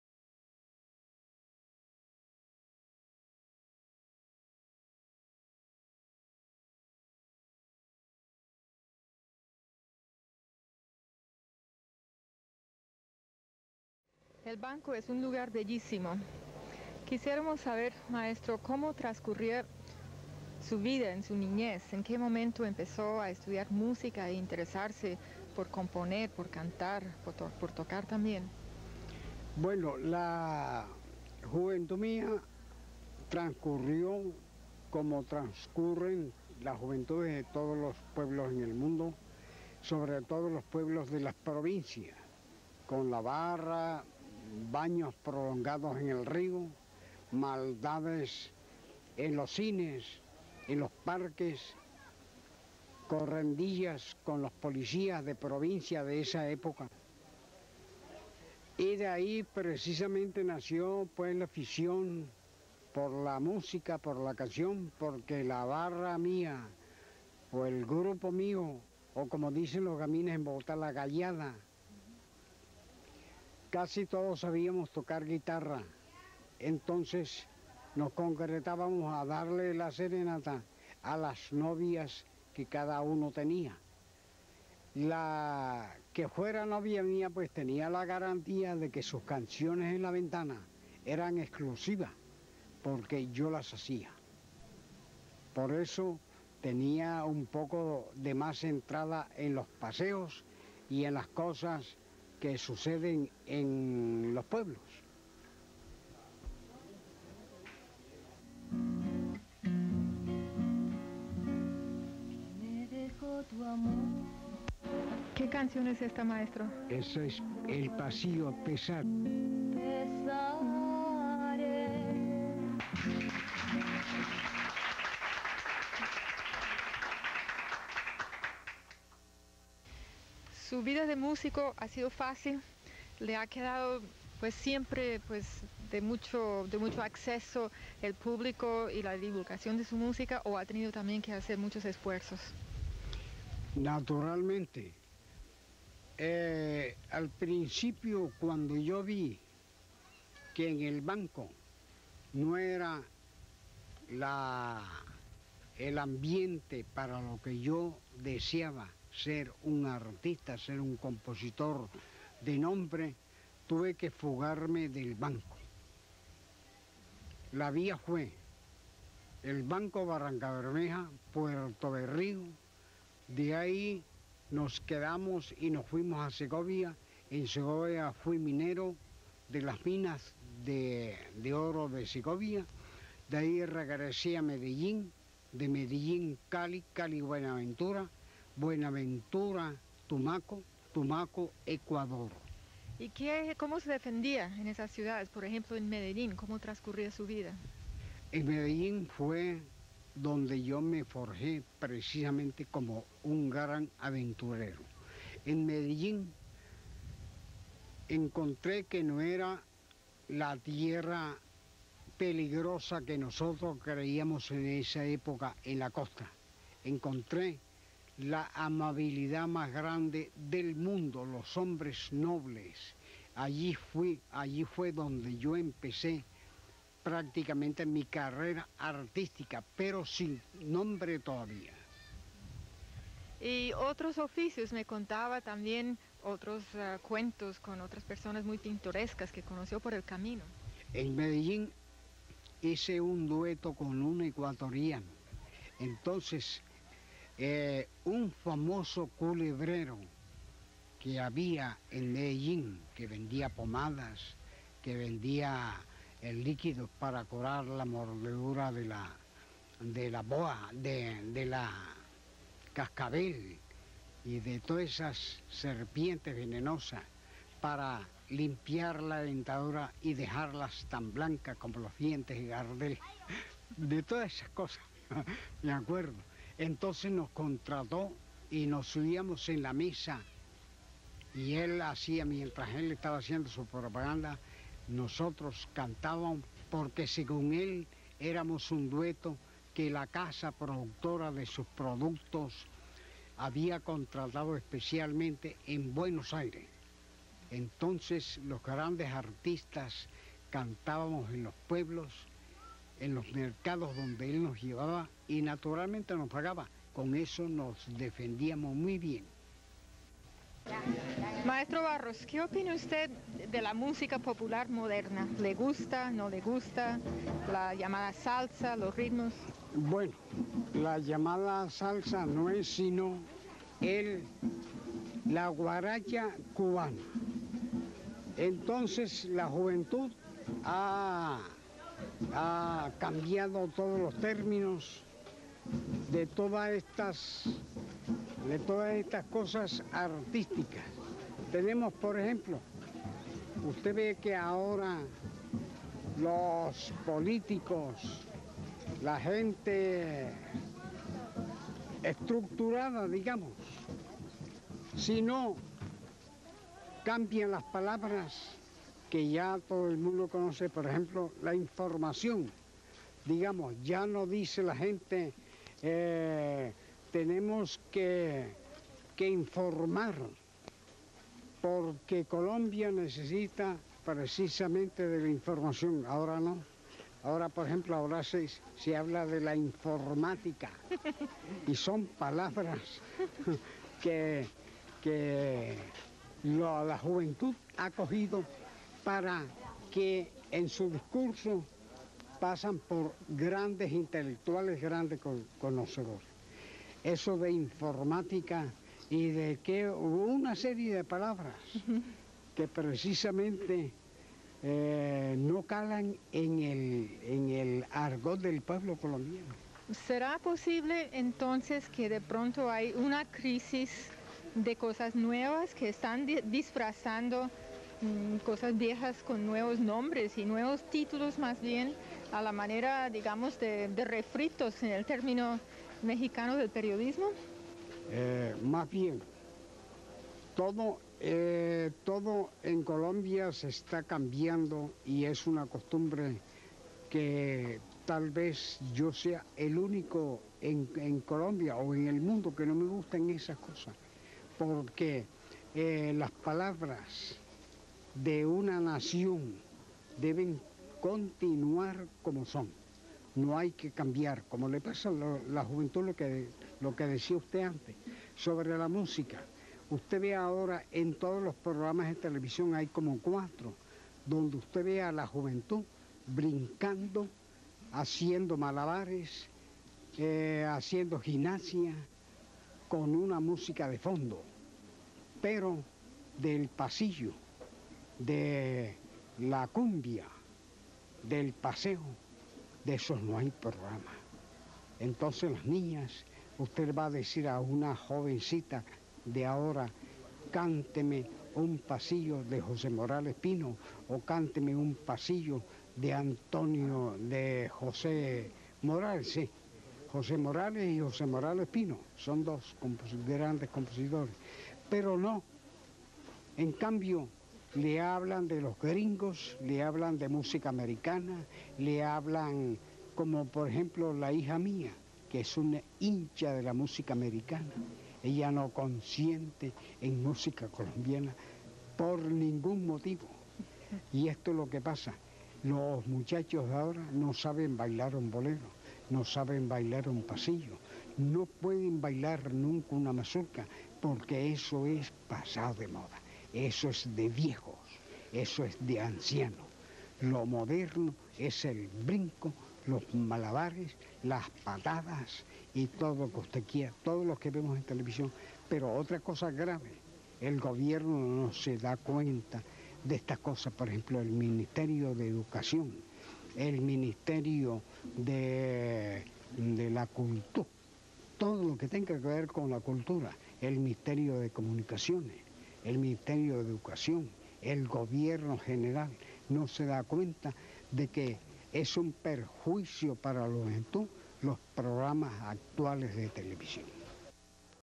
José Benito Barros, la cumbia en El Banco, Magdalena, III parte, 1985
Imágenes del documental dedicado al maestro, José Benito Barros, creador de cientos de composiciones del cancionero popular colombiano. Barros Palomino nació en El Banco, Magdalena, el 21 de marzo de 1915. Desde allí exaltó la cumbia y las raíces vernáculas del folclor colombiano. Escribió canciones en tiempo de bolero, currulao, vallenato, pasillo, cumbia. Recordado también como creador del Festival de la Cumbia, para apoyar a los compositores musicales y a los exponentes dancísticos. Murió el 12 de mayo de 2007.